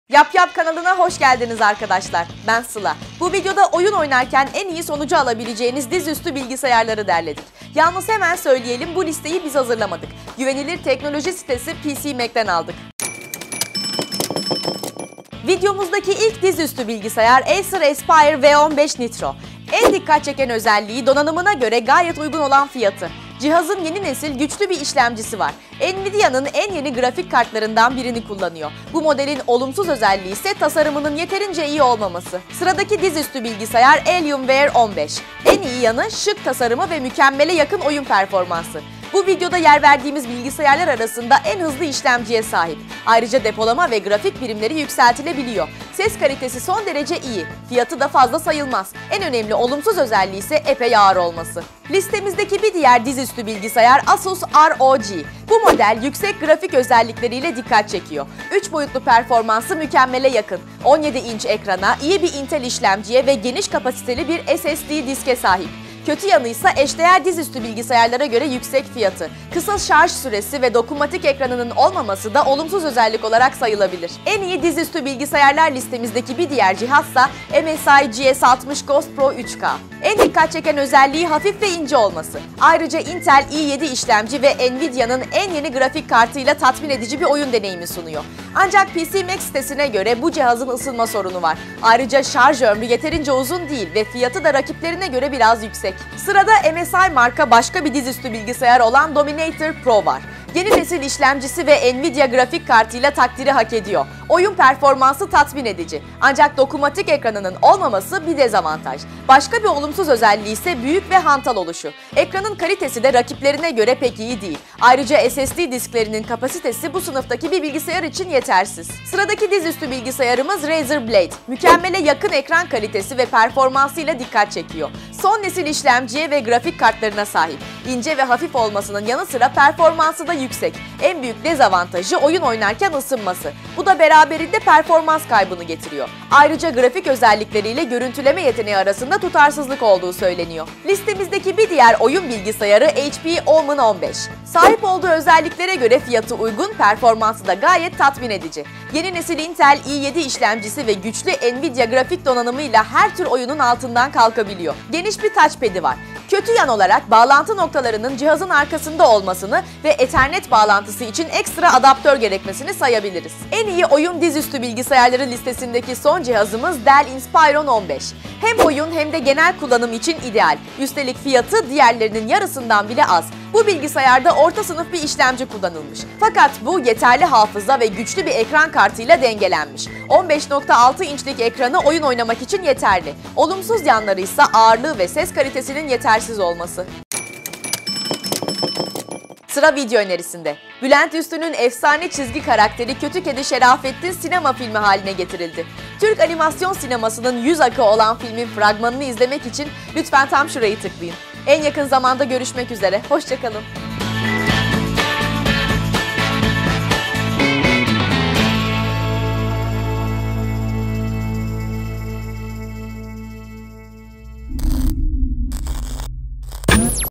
En İyi 10 Dizüstü Oyun Bilgisayarı
Bu videoda, oyun oynarken en iyi sonucu alabileceğiniz dizüstü bilgisayarları derledik. Yalnız hemen söyleyelim, bu listeyi biz hazırlamadık, güvenilir teknoloji sitesi PCmag'den  aldık.

Videomuzdaki ilk dizüstü bilgisayar "Acer Aspire V-15 Nitro". En dikkat çeken özelliği, donanımına göre gayet uygun olan fiyatı. Cihazın yeni nesil, güçlü bir işlemcisi var. Nvidia’nın en yeni grafik kartlarından birini kullanıyor. Bu modelin olumsuz özelliği ise tasarımının yeterince iyi olmaması.

Sıradaki dizüstü bilgisayar, "Alienware 15". En iyi yanı şık tasarımı ve mükemmele yakın oyun performansı. Bu videoda yer verdiğimiz bilgisayarlar arasında en hızlı işlemciye sahip. Ayrıca depolama ve grafik birimleri yükseltilebiliyor. Ses kalitesi son derece iyi. Fiyatı da fazla sayılmaz. En önemli olumsuz özelliği ise epey ağır olması.

Listemizdeki bir diğer dizüstü bilgisayar, Asus ROG. Bu model yüksek grafik özellikleriyle dikkat çekiyor. 3 boyutlu performansı mükemmele yakın. 17 inç ekrana, iyi bir Intel işlemciye ve geniş kapasiteli bir SSD diske sahip. Kötü yanı ise eşdeğer dizüstü bilgisayarlara göre yüksek fiyatı. Kısa şarj süresi ve dokunmatik ekranının olmaması da olumsuz özellik olarak sayılabilir.

En iyi dizüstü bilgisayarlar listemizdeki bir diğer cihazsa, "MSI GS60 Ghost Pro 3K". En dikkat çeken özelliği hafif ve ince olması. Ayrıca Intel i-7 işlemci ve Nvidia’nın en yeni grafik kartı ile tatmin edici bir oyun deneyimi sunuyor. Ancak, PCmag sitesine göre, bu cihazın ısınma sorunu var. Ayrıca şarj ömrü yeterince uzun değil ve fiyatı da rakiplerine göre biraz yüksek.

Sırada MSI markalı başka bir dizüstü bilgisayar olan "Dominator Pro"  var. Yeni nesil işlemcisi ve Nvidia grafik kartıyla takdiri hak ediyor. Oyun performansı tatmin edici, ancak dokunmatik ekranının olmaması bir dezavantaj. Başka bir olumsuz özelliği ise büyük ve hantal oluşu. Ekranın kalitesi de rakiplerine göre pek iyi değil. Ayrıca SSD disklerinin kapasitesi bu sınıftaki bir bilgisayar için yetersiz.

Sıradaki dizüstü bilgisayarımız Razer Blade, mükemmele yakın ekran kalitesi ve performansıyla dikkat çekiyor. Son nesil işlemciye ve grafik kartlarına sahip. İnce ve hafif olmasının yanısıra performansı da yüksek. En büyük dezavantajı, oyun oynarken ısınması. Bu da beraberinde performans kaybını getiriyor. Ayrıca grafik özellikleri ile görüntüleme yeteneği arasında tutarsızlık olduğu söyleniyor.

Listemizdeki bir diğer oyun bilgisayarı, "HP Omen 15". Sahip olduğu özelliklere göre fiyatı gayet uygun. Performansı da gayet tatmin edici. Yeni nesil Intel i-7 işlemcisi ve güçlü Nvidia grafik donanımıyla her tür oyunun altından kalkabiliyor. Geniş bir touchpad’i var. Kötü yan olarak, bağlantı noktalarının cihazın arkasında olmasını ve ethernet bağlantısı için ekstra adaptör gerekmesini sayabiliriz.

En iyi oyun dizüstü bilgisayarları listesindeki son cihazımız, "Dell Inspiron 15". Hem oyun hem de genel kullanım için ideal bir cihaz. Üstelik fiyatı diğerlerinin yarısından bile az. Bu bilgisayarda orta sınıf bir işlemci kullanılmış. Fakat bu, yeterli hafıza ve güçlü bir ekran kartıyla dengelenmiş. 15 nokta 6 inçlik ekranı oyun oynamak için yeterli. Olumsuz yanlarıysa, ağırlığı ve ses kalitesinin yetersiz olması.